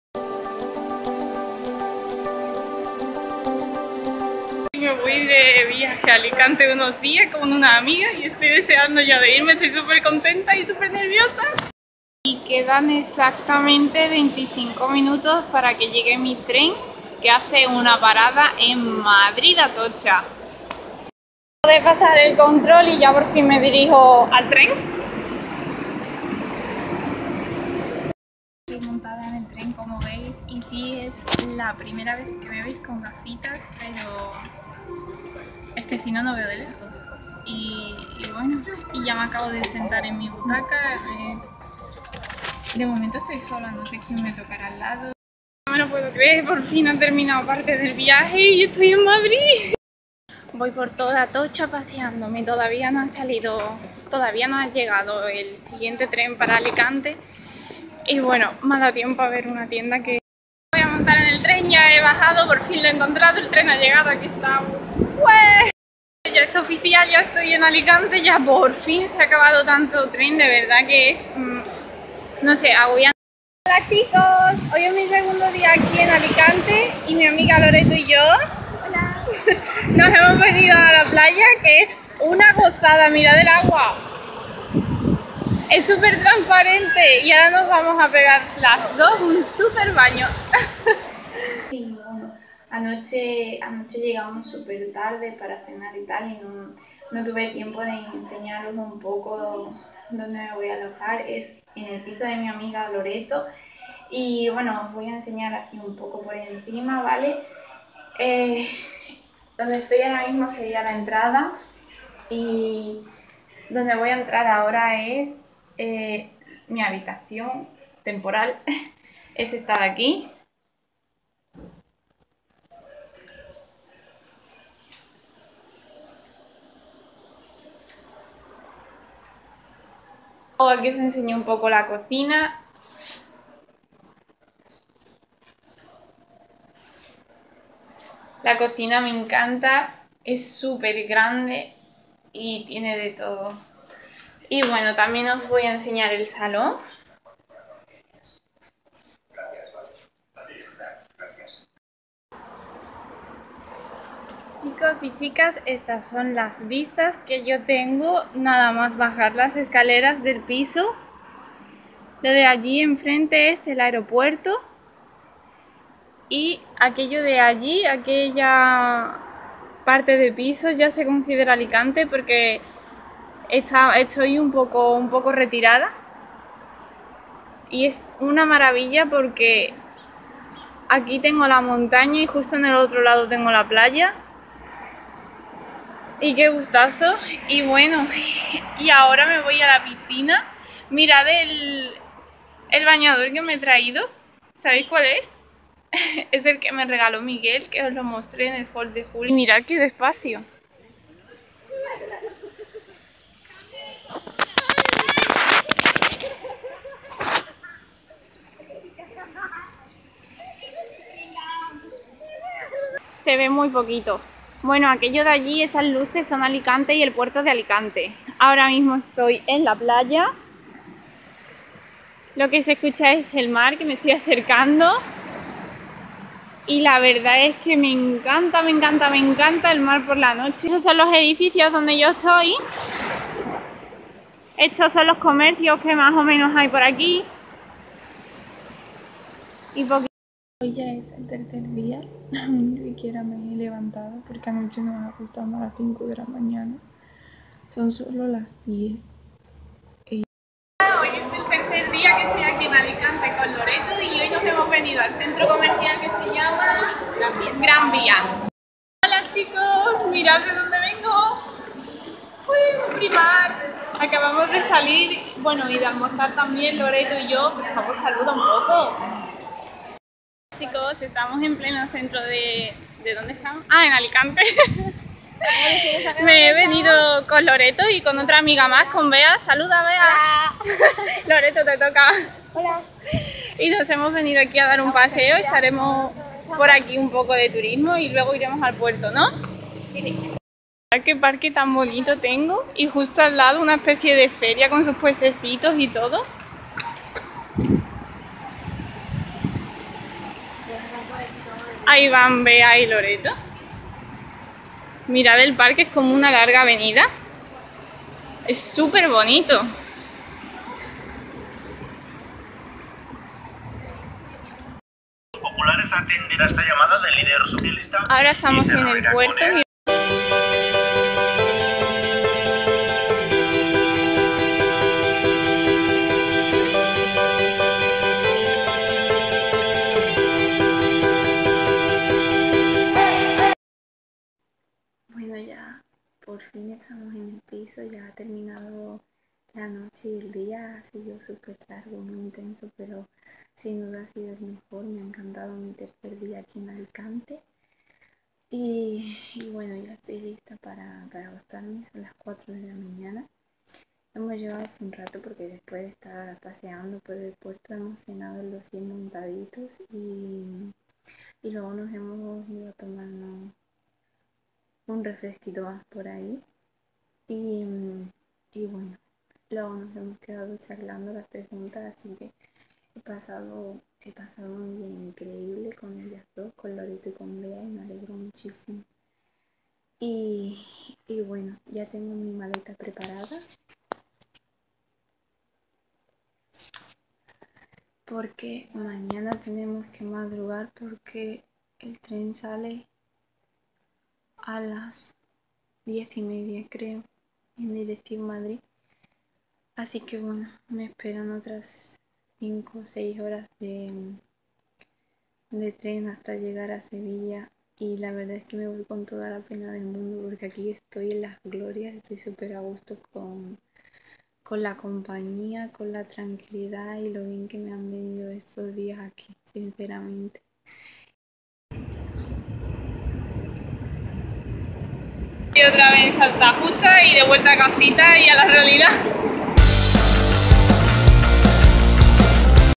Viaje por Alicante. Días de chicas! 💄🍕 👭💅
Hoy os traigo un vlog dedicado a Alicante ya que me he ido allí unos días,a casa de una amiga. Han sido unas maravillosas vacaciones que quiero compartir con vosotr@s! 💋